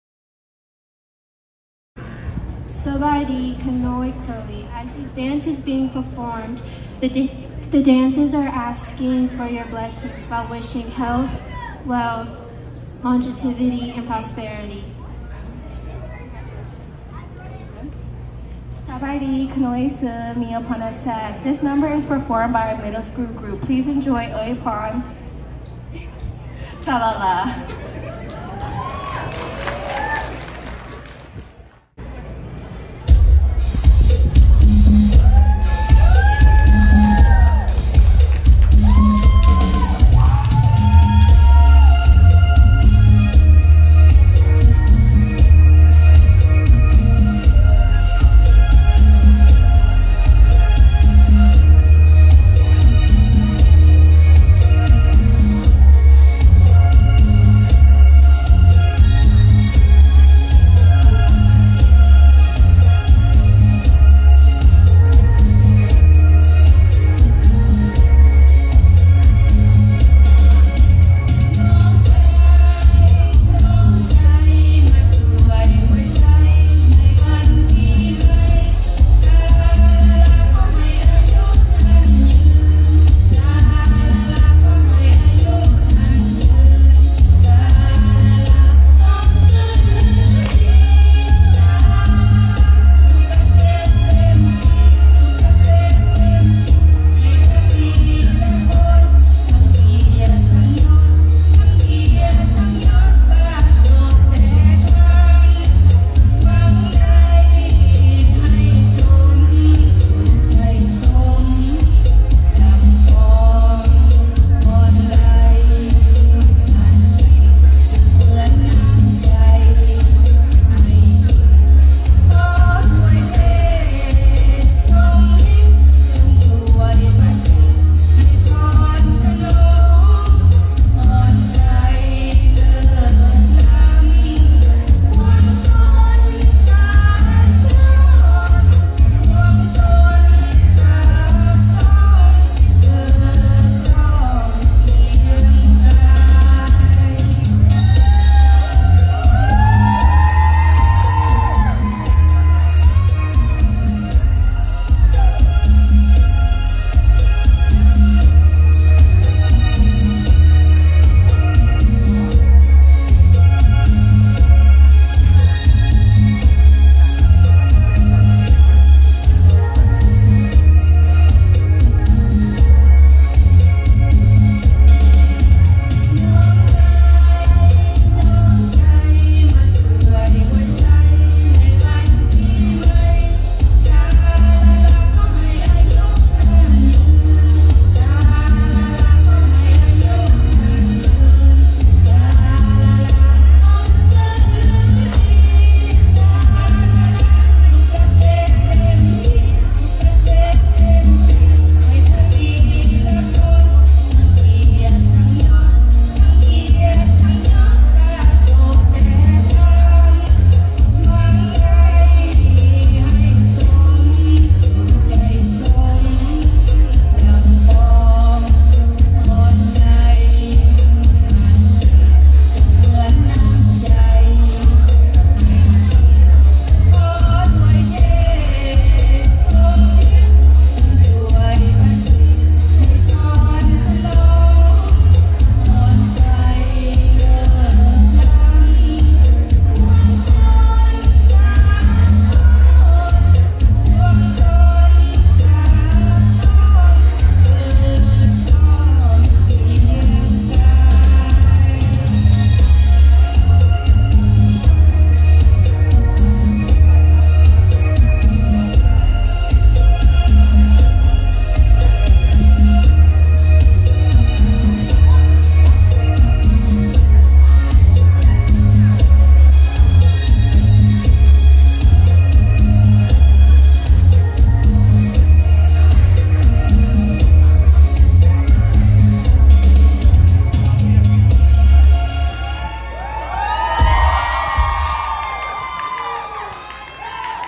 “Ouy Pone Tala” dance by Middle Group of Royal Lao Classical Dancers Middle Tennessee
Asian Traditional Dance. November 11, 2023. Royal Lao Classical Dancers presents A Night In Laos fundraiser Gala.  The event included amazing performances by our youth and all the way up to young adults. Authentic Lao cuisine was served as guests dined among the beautifully decorated venue.  This black tie event benefits the continued education of our Lao heritage. Thank you to the organizers and many volunteers to help showcase the beauty of Lao culture.

To support this tradition, go to rlcdtn.org

Choreographers/Senior Advisors/ Founders:
(Hair and Wardrobe)
Sonnikhom Vannachith
Thassany Pothikan
Bangthong Chindavanh

Instructors:
(Hair and Wardrobe)
Jenni Chindavanh
Sio Vankham-Saengchanh
Damone Xaysongkham
Lea Saen
Rebekah Sithsayavong
Pany Akhom

Make-up:
Cindy Rajvongs
Felicia Dao Morklithavong
Malena Mouthouta
Delilah Niravong

Manager/Supervisor/MVP:
Onn Aphayavong

Photographers:
Joe Phimvong
Souphaphonh Kounlavong

Video-ography:
Vinny Nouanemany
Seua Tiger Chanthaphanij
Sam Kanjanabout

Event help:
Florals: Deun Tran Dao Ha
Backdrops: Willow Events / Jimmy Sengthavorn/ Flashing Stars Photo Booth
Lotus flowers: RLCD Instructors and Lanoi Hood
Team: Nini White Kaylin Thomas Anna Chamleunsouk Stephanie Somz Darrika Morklithavong
Band: Fah Saii
Traditional Band: Kinnaly Banouvong Bounmy Vongkhamchanh Bounchan Boutsady
Set up: The Grazing Mama, LLC
Food items:
Phaivanh Kounlavong Somsy Saenboutarath Chai Souksavong Vandy Phimavong Bounkeo Robert Manola Saenphansiri Khammouan Boupharath Phonthip Ratsavong Kethnalone Chanthavatdy Keil Chindavanh Chansamone Chindavahn Diane Siharath Khonsavanh Manivong Deun Manivong Chansanith Manivong Thai Kitchen Lee Sivilay Maly Phouangkeo-Akaraborworn  Boualay Sayasack

Fruit Table: Wut Phom

Bar: Nikkie Fane Sylus Chindavanh Jalen Xaysongkham

Vouchers/PhotoBooth:
Nancy Siharath Melody Souvannasane Gray Jimmy Dam Destiny Siharath